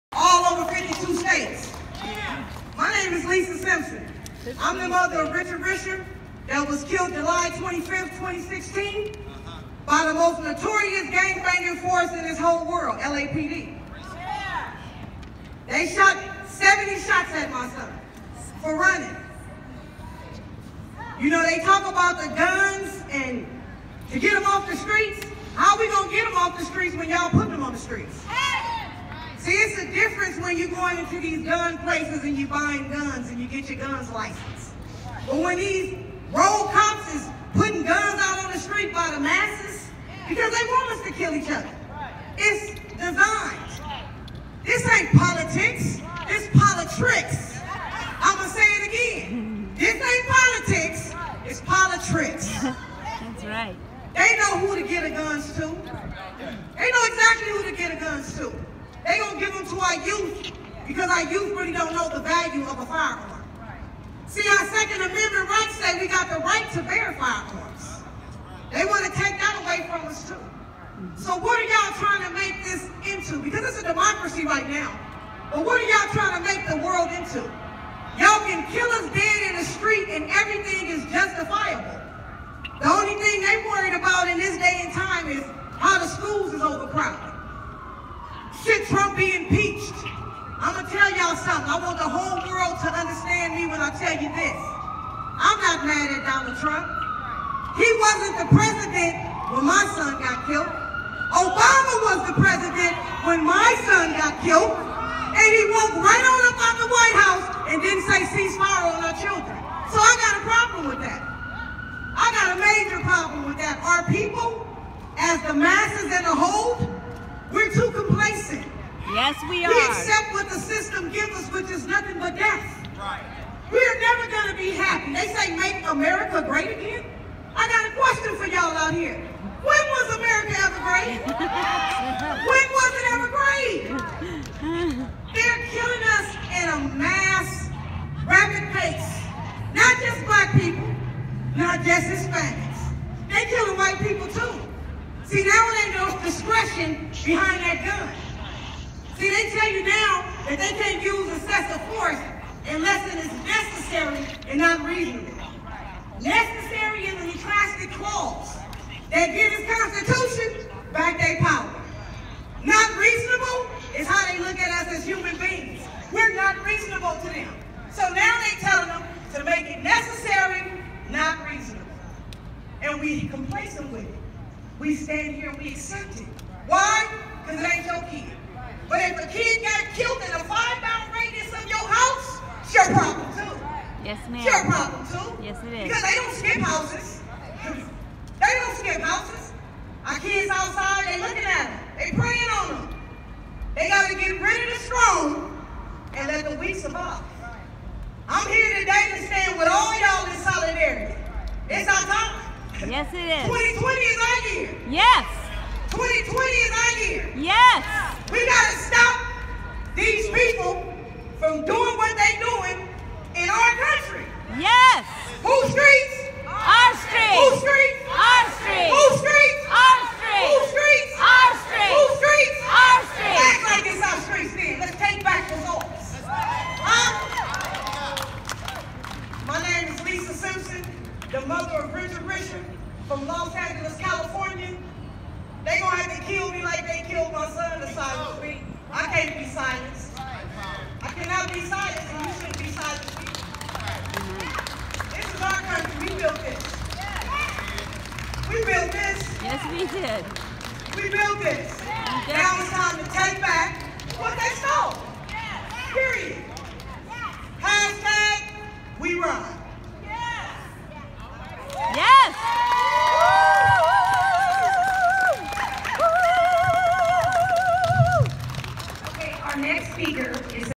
LISA SIMPSON (Part 2of2)StopPoliceBrutality DeEscalateOhio HeartbeatMovement Rally 9/18/19
POWERFUL CALL TO ACTION by Lisa Simpson - Columbus Ohio, 9/18/19 DoSomething, a gathering of survivors of gun/police violence and faith based groups to demand strong laws to protect Ohio from the escalating gun violence in our State.
Lisa Simpson came from Los Angeles, California to stand in solidarity to Make Change Now!